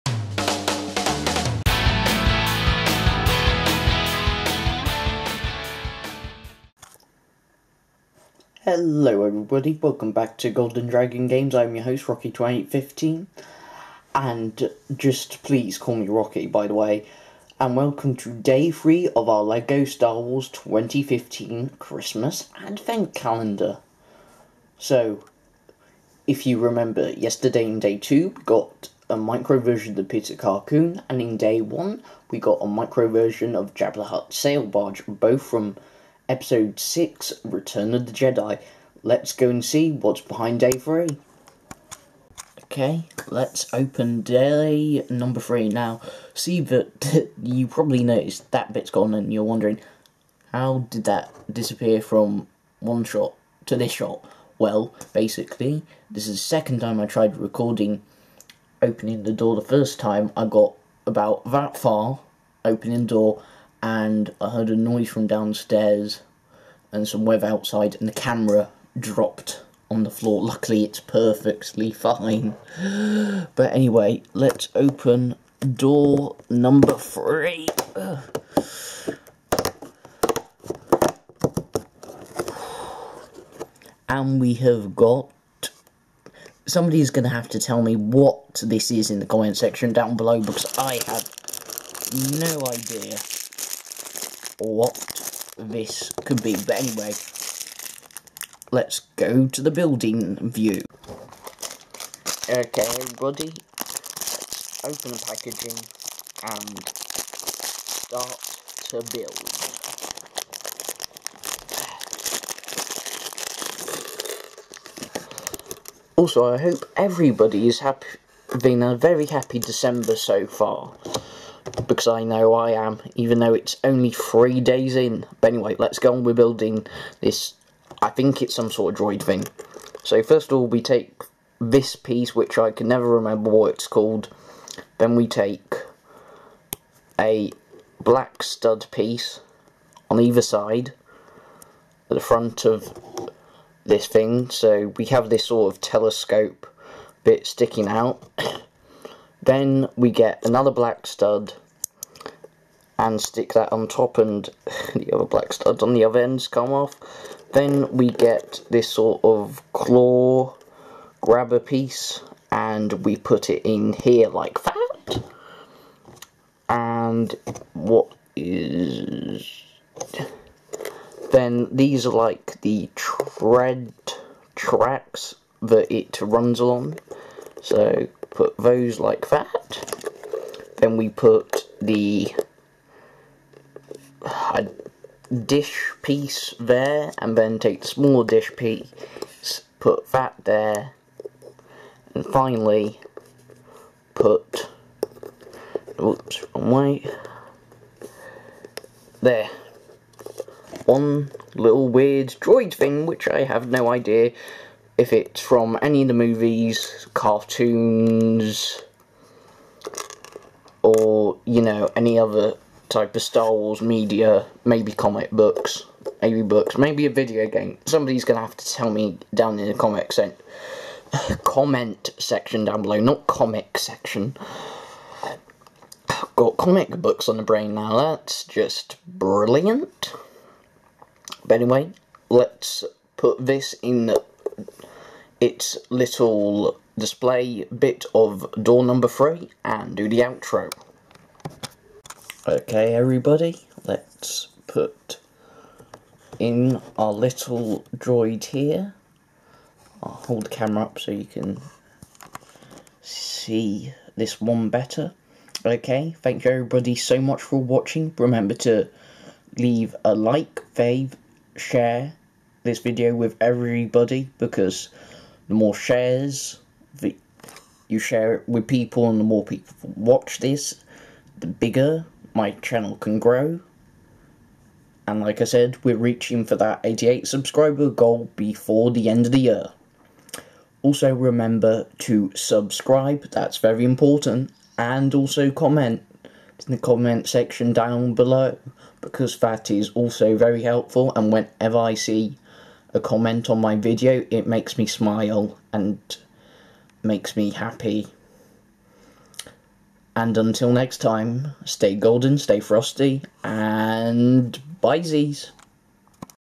Star Wars Lego's Advent Calendar Day,3
(day 3) Hello everyone Rocky here its that time of year again it Christmas so time for the 24 days of GoldenMus get it it a pun on Xmas never mind that was a terrible pun but i hope you all enjoy remember to leave a like,fav,share the video,comment,and subscribe till next time stay golden stay frosty and BBBBYYYYEEEEZZZZSSSS *^-^*